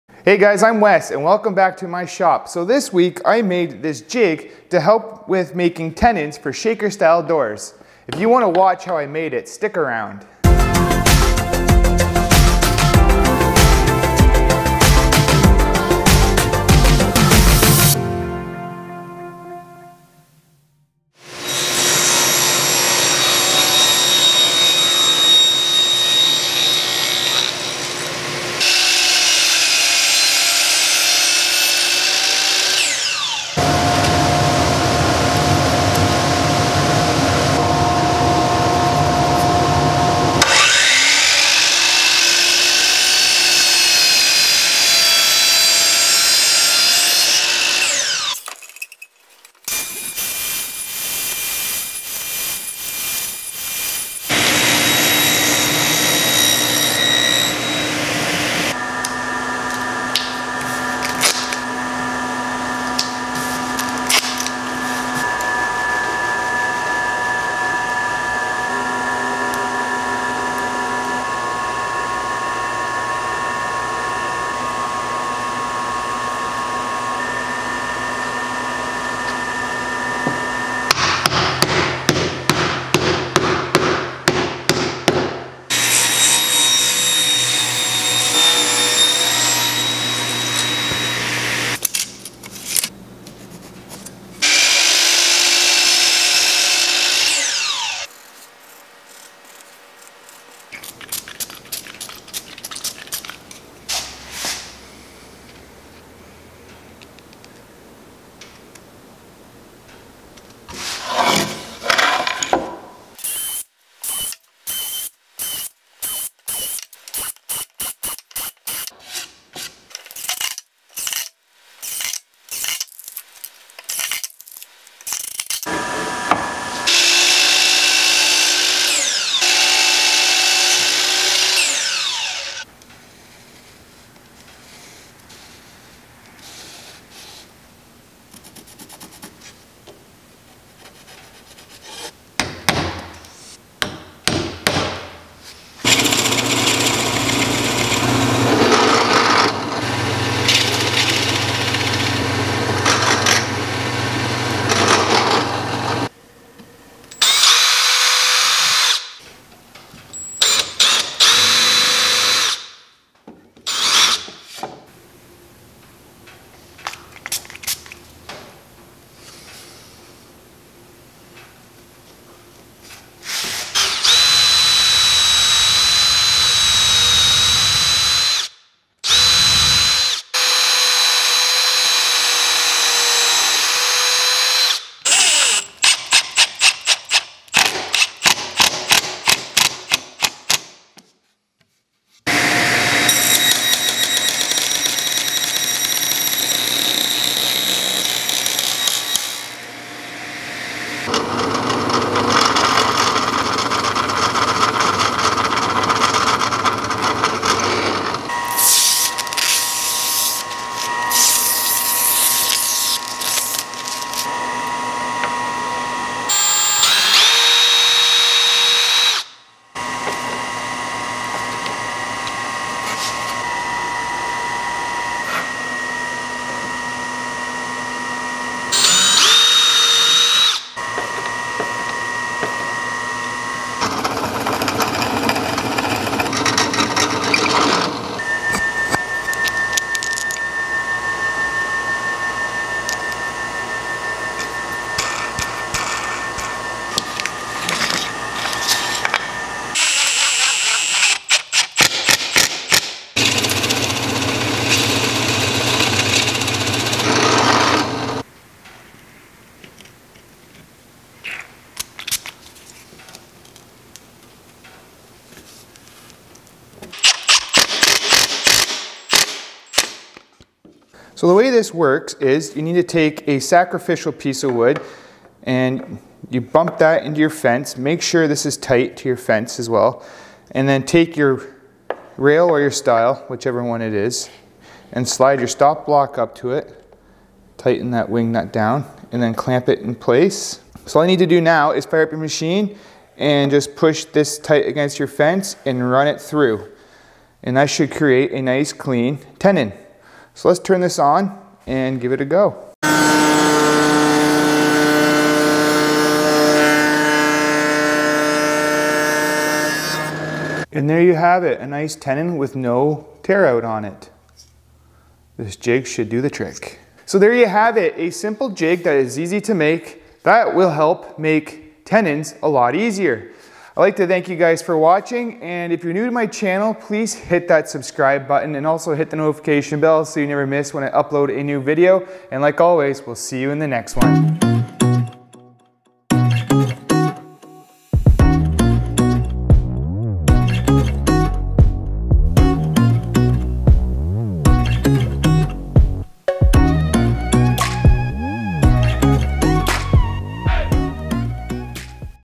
Router Table Tenon Jig
In this video I show you how I made a jig to help get perfect tenons for shaker style doors on the router table.